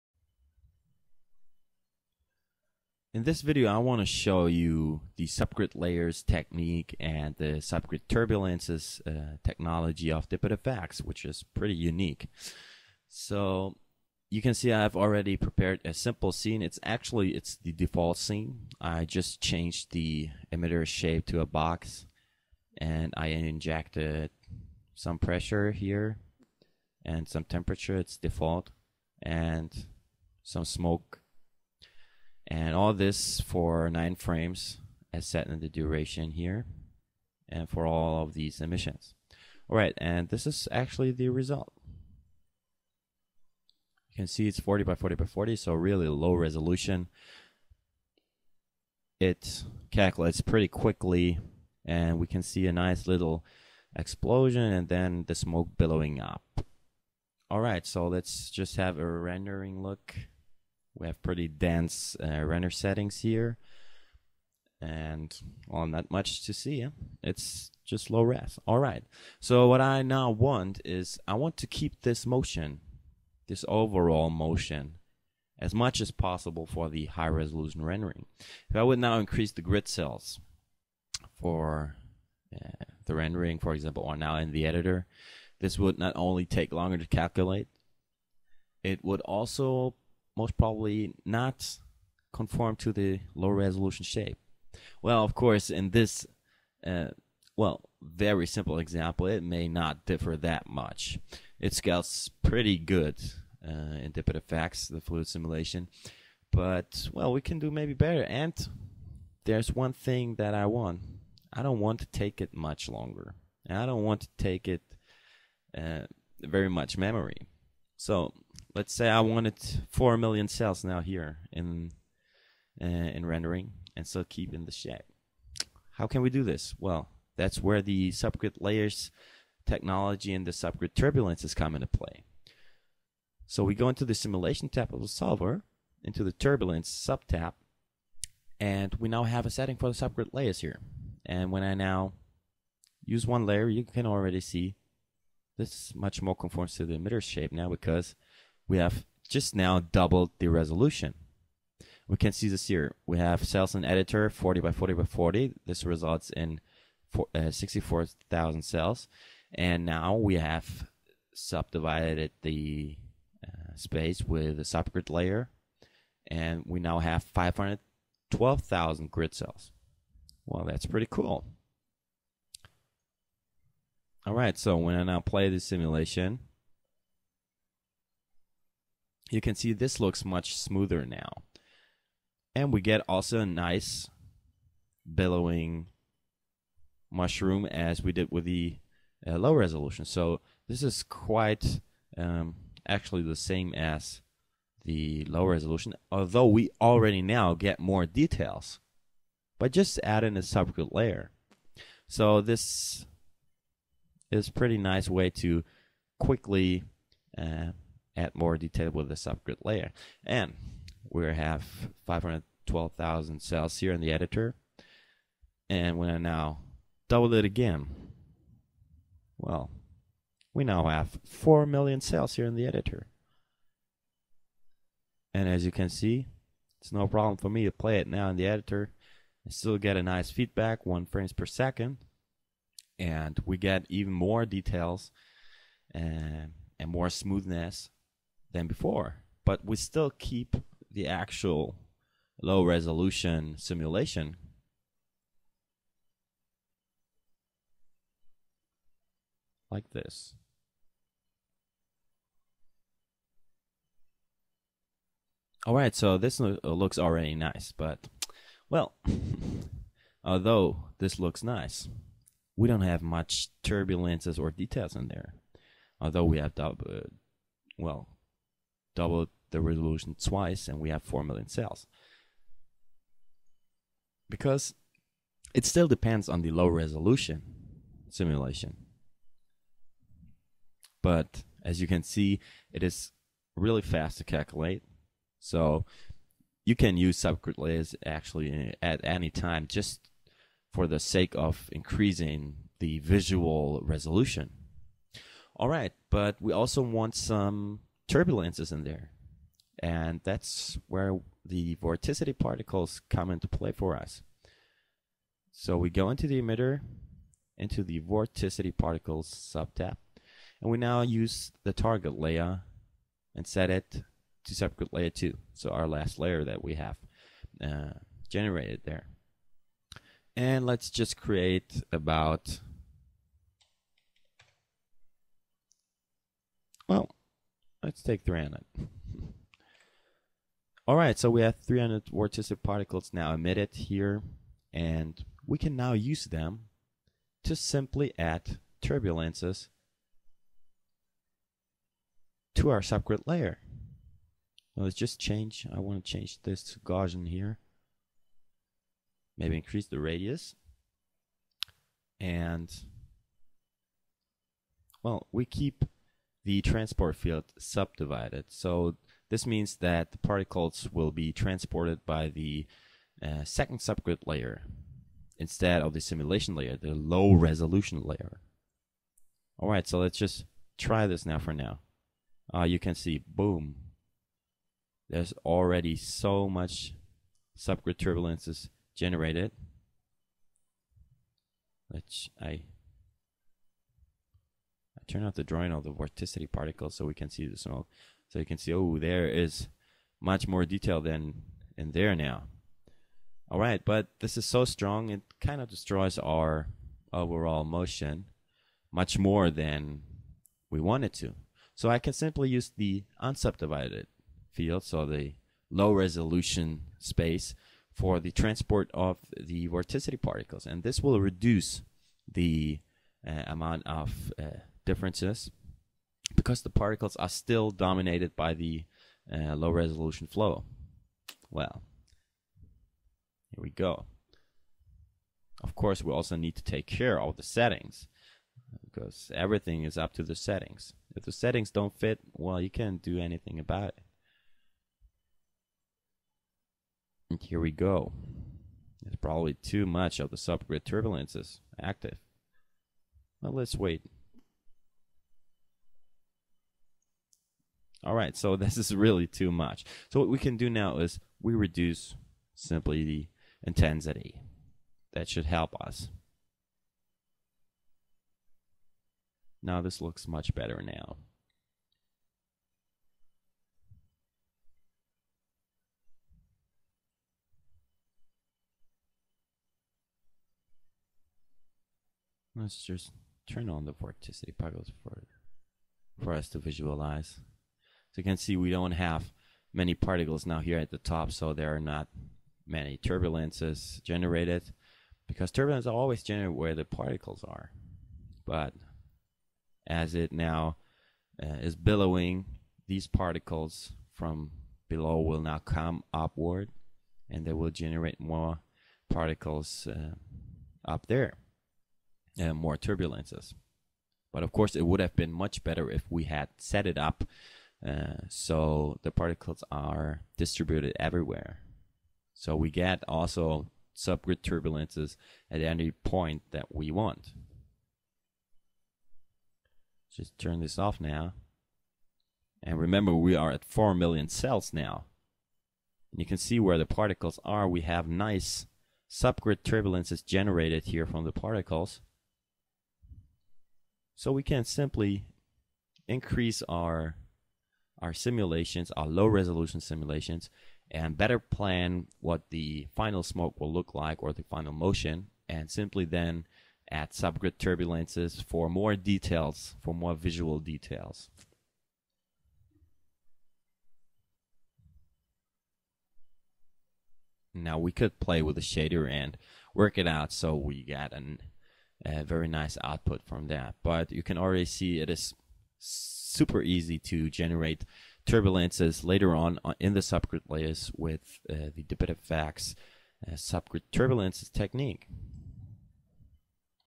DPIT Subgrids Basics
Basics on how to work with Subgrid layers and subgrid turbulences in DPIT Effex (Effex 1.0 but still applies to 1.5)